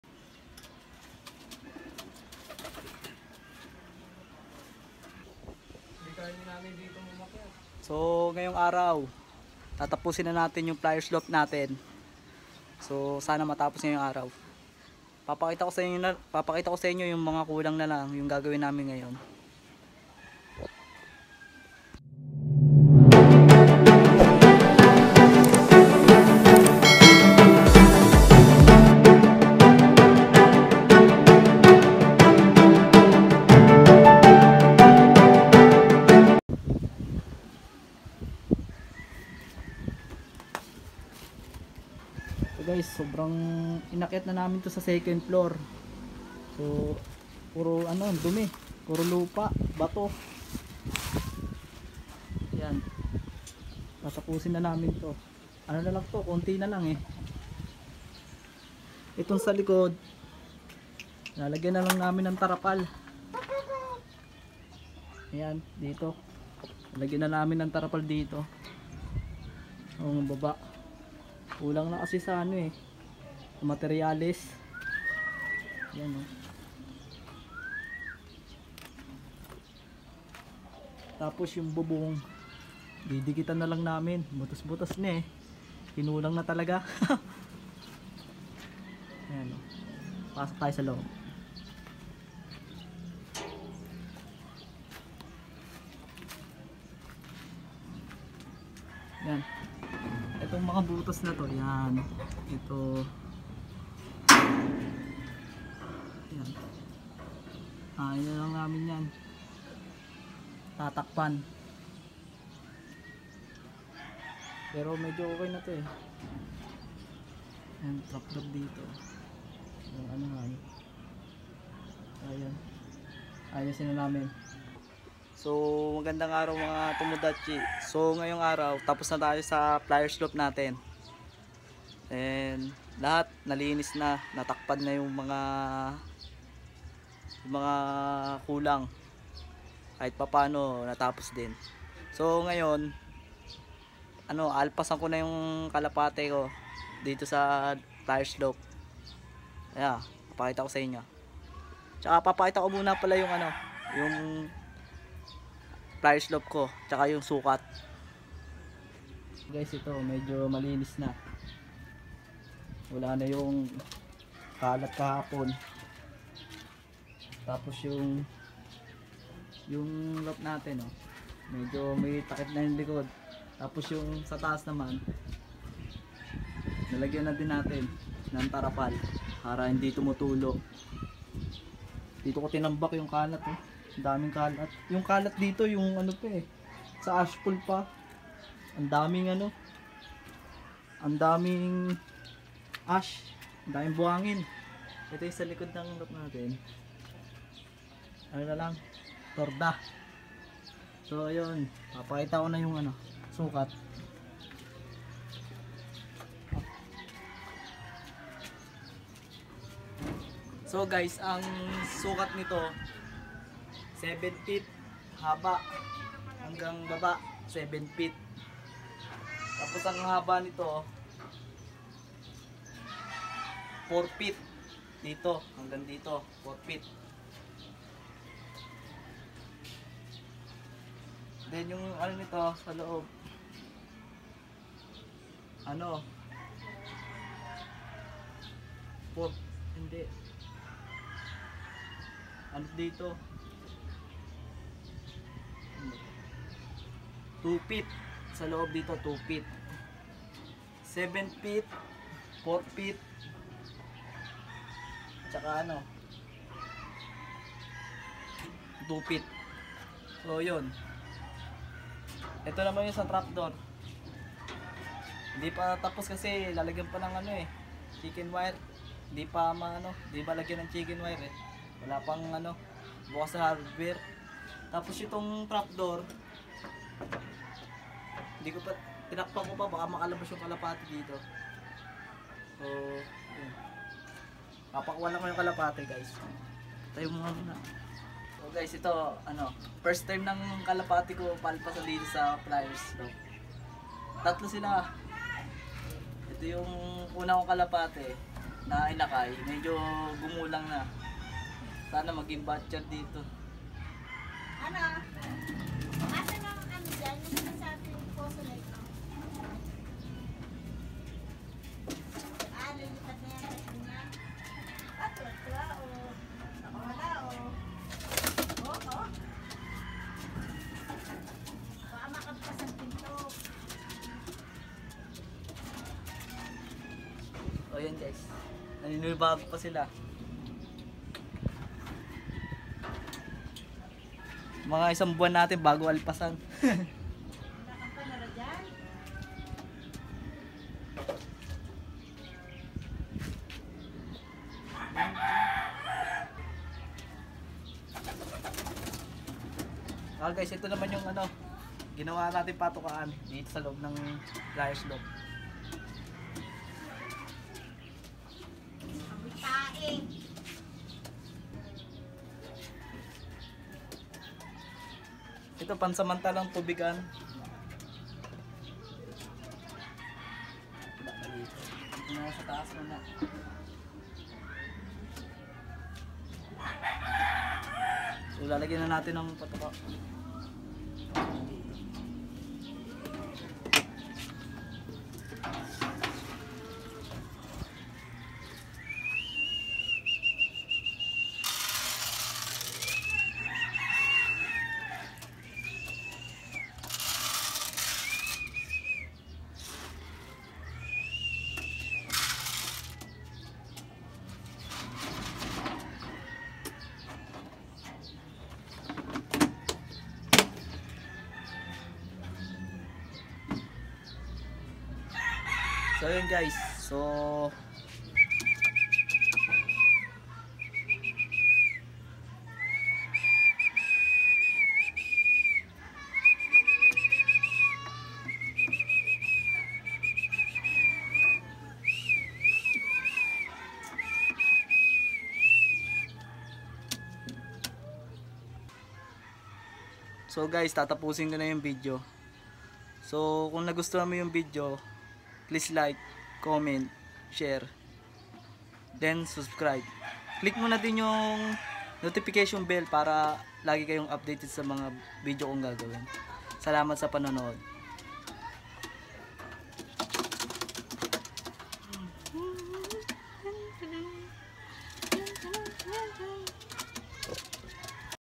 TAPOS NA FLYERS LOFT NAMIN | UNANG KALAPATI SA FLYER LOFT 3 PALANG
Height = 7ft
Width =4 ft
Length = 6ft

Newbie about sa mga kalapati :)